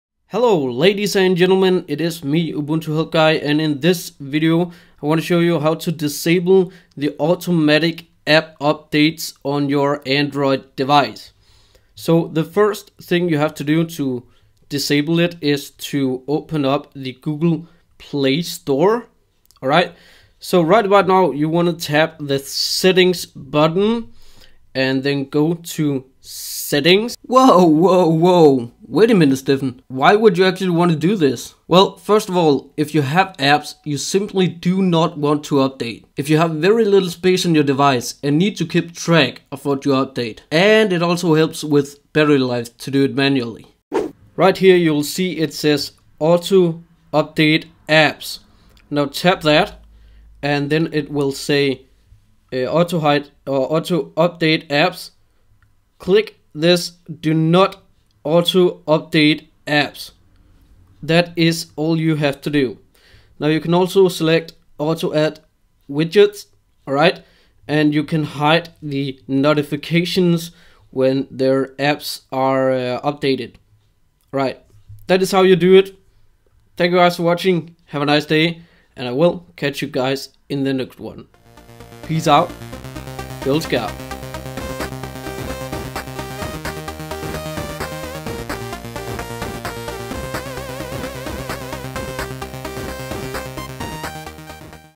Android Tip - Disable Auto App Updates - Jelly Bean 4.3
Today, I'm showing you how to disable the automatic app updates on your Android 4.3 Jelly Bean device. If you liked the video, remember to click the like-button!